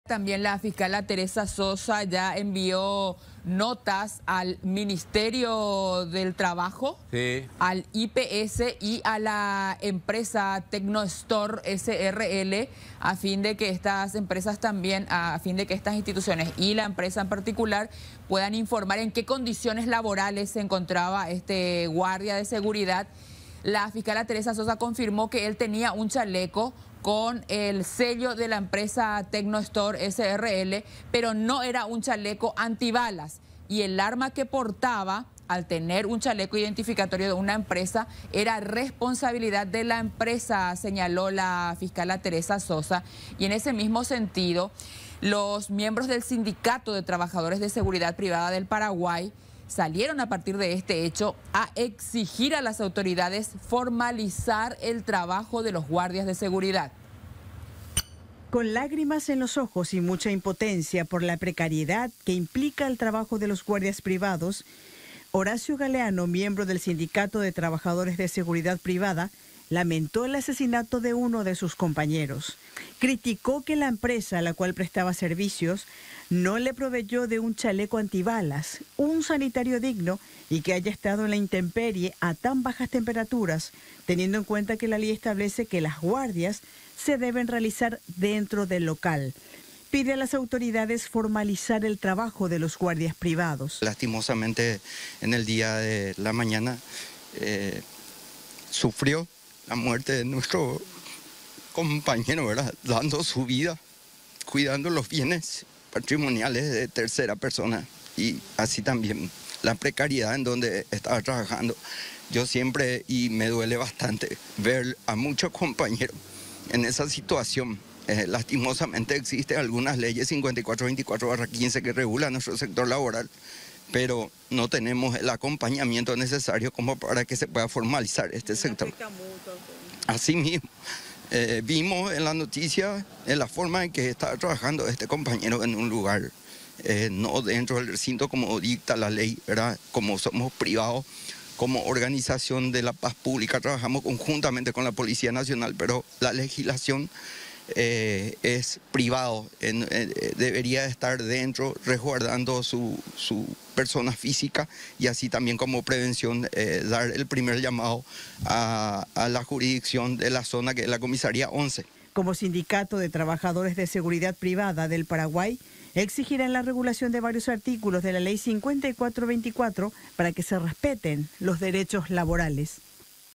Muerte de un guardia desnuda precariedad laboral
Con lágrimas en los ojos y mucha impotencia Horacio Galeano, miembro del Sindicato de Trabajadores de Seguridad Privada, lamentó el asesinato de uno de sus compañeros y exigió a las autoridades formalizar el trabajo de los guardias.